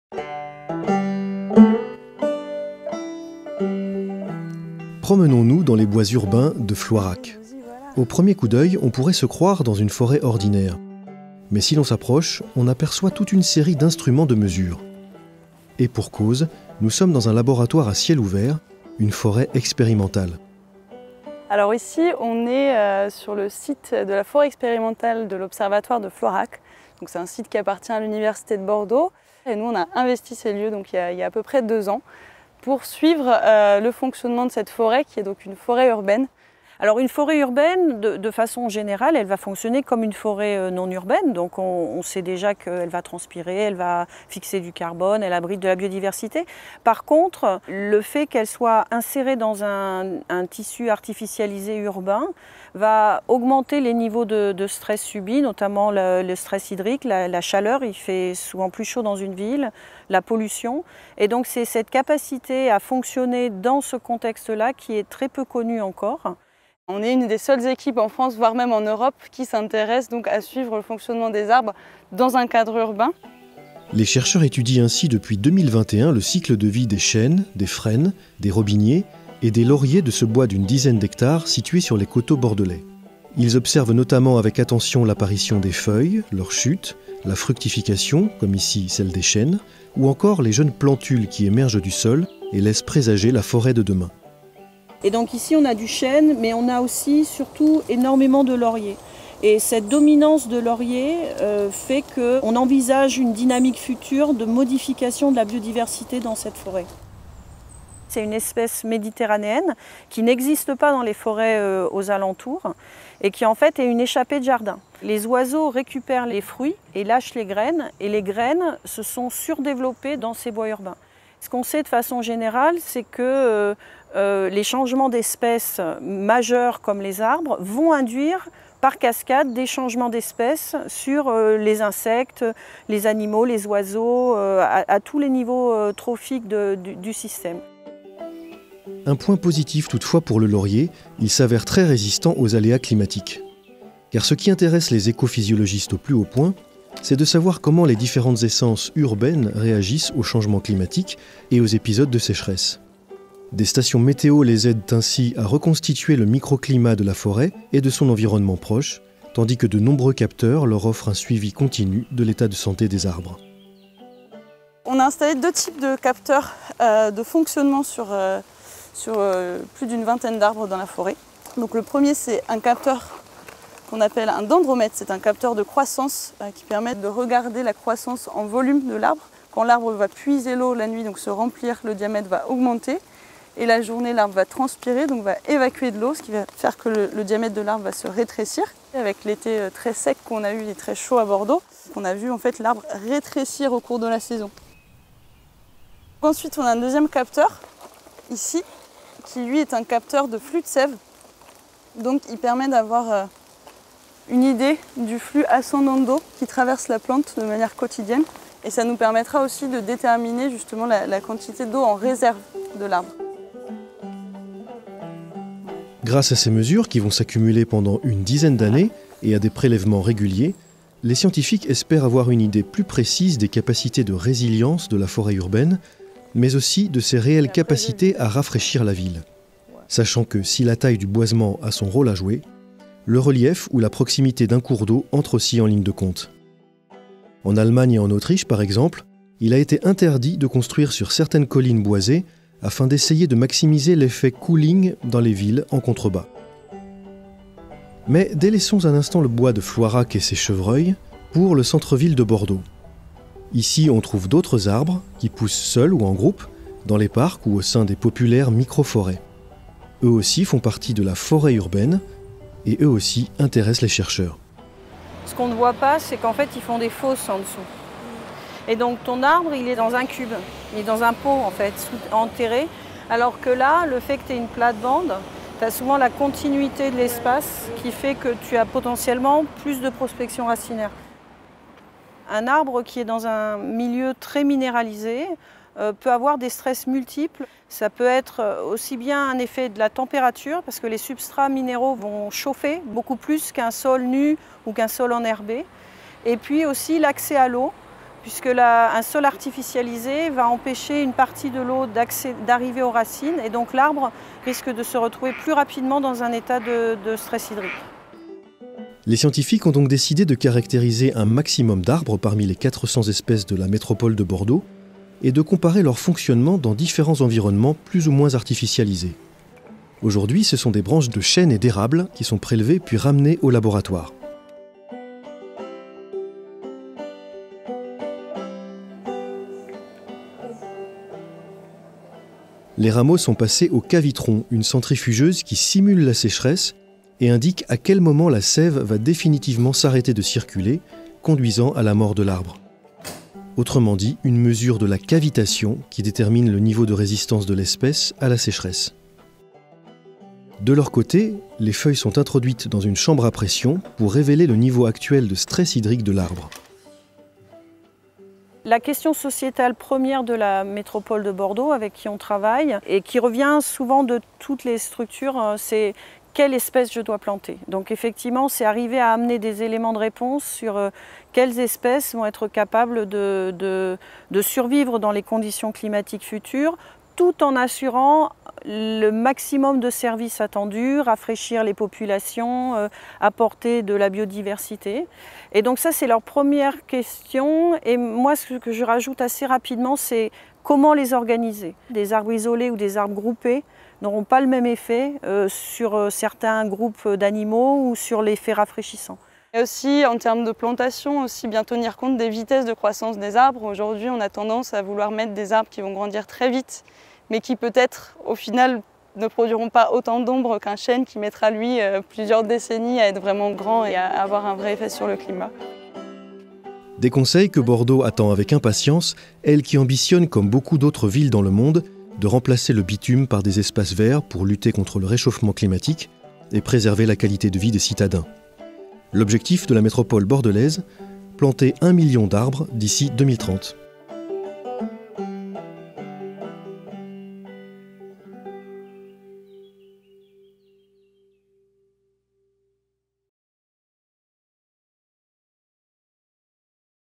Une forêt urbaine expérimentale à Bordeaux pour guider les futures plantations d'arbres | Reportage
Après des décennies de bétonisation et de plans d’urbanisation dédiés à la voiture, la ville entend se (re) verdir. Des micro-forêts expérimentales sont actuellement à l’étude à Floirac, en périphérie de Bordeaux. Croissance des arbres, fructification, stress hydrique, quantité de sève, évolution de la biodiversité... Les écophysiologistes étudient l'évolution de différentes essences d'arbres afin d'éclairer les choix futurs en matière de plantation. Dans un contexte de réchauffement climatique, les arbres joueront en effet un rôle majeur pour rafraîchir la ville de demain.

Réalisation : Jean-Pierre Courbatze
Journalisme : Laurianne Geffroy
Production : Universcience 2023